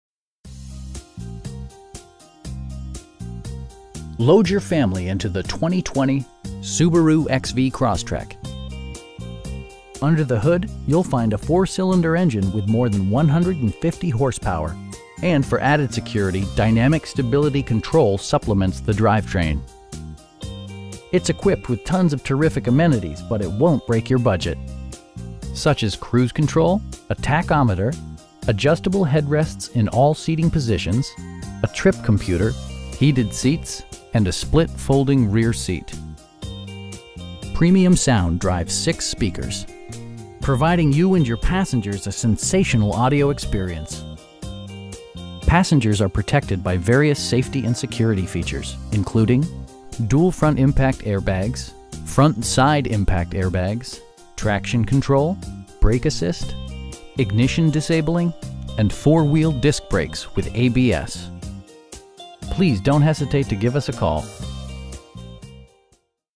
2020 Subaru Crosstrek 2.0i Premium
Load your family into the 2020 Subaru XV Crosstrek. Under the hood you'll find a 4 cylinder engine with more than 150 horsepower, and for added security, dynamic Stability Control supplements the drivetrain. It's equipped with tons of terrific amenities, but it won't break your budget. Such as cruise control, a tachometer, adjustable headrests in all seating positions, a trip computer, heated seats, air conditioning, power windows, and a split folding rear seat. Premium sound drives 6 speakers, providing you and your passengers a sensational audio experience. Passengers are protected by various safety and security features, including: dual front impact airbags, front side impact airbags, traction control, brake assist, ignition disabling, and 4 wheel disc brakes with ABS. We pride ourselves in consistently exceeding our customer's expectations. Please don't hesitate to give us a call.